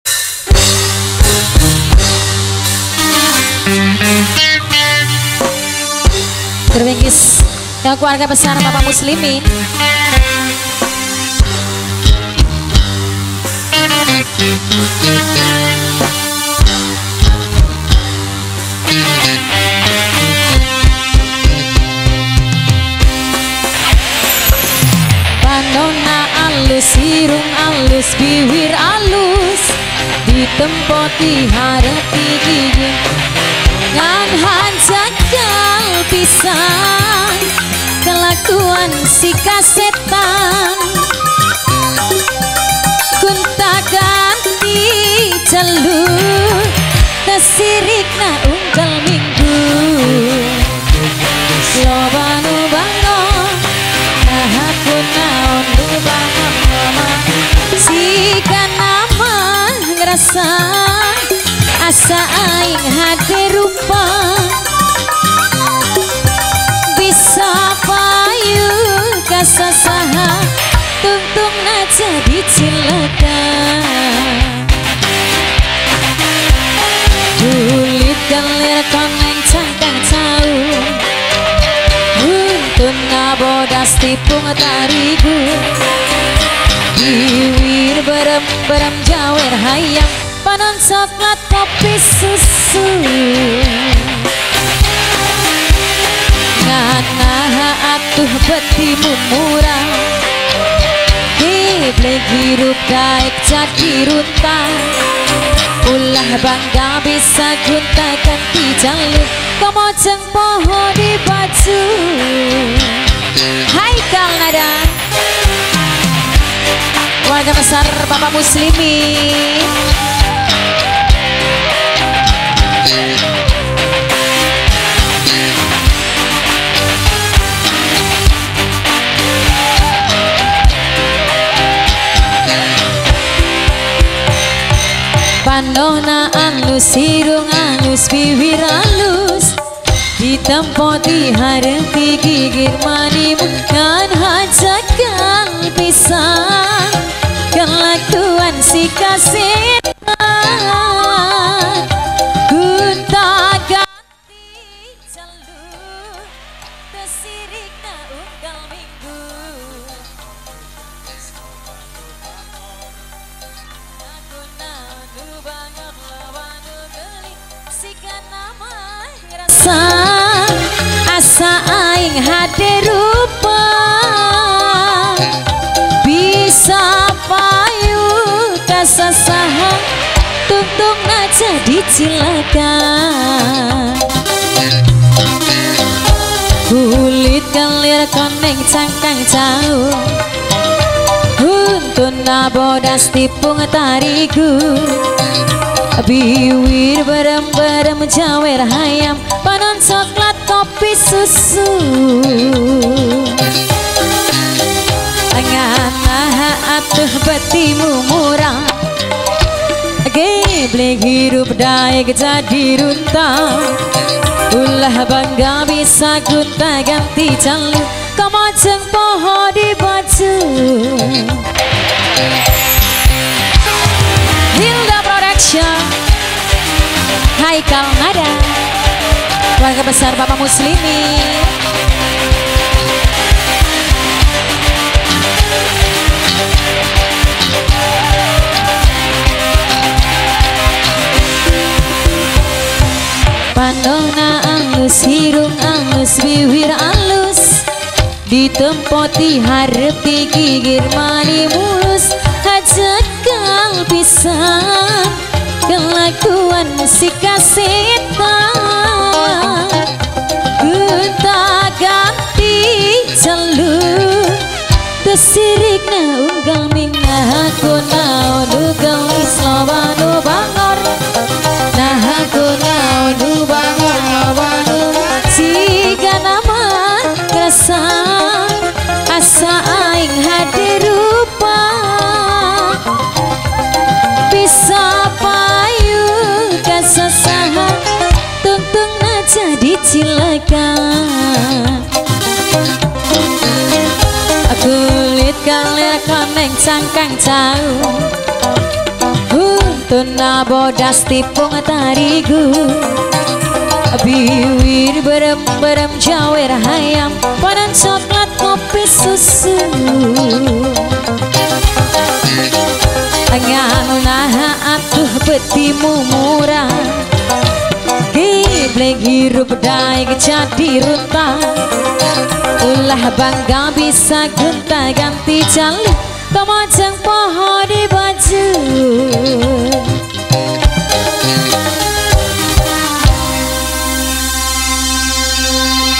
TERBARU 2022 BARONGSAI // LIVE SHOW SING DANGDUT HAIKAL NADA EDISI SENGKON SENIN, 7 NOPEMBER 2022
LIVE SHOW SINGA  DANGDUT " HAIKAL NADA " PIMP : BAPAK SOBIDIN ALAMAT : DESA AMBULU LOSARI CIREBON HP/WA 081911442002 DALAM RANGKA
RESEPSI KHITANAN ANANDA : CACA MAULANA PUTRA BAPAK MUSLIMIN IBU DARTI GG. PANCAKERTI RT. 03/RW. 03 DESA LEMAHABANG KEC. TANGJUNG KAB. BREBES
PELAKSANAAN, SENIN, 7 NOPEMBER 2022

Haikal Nada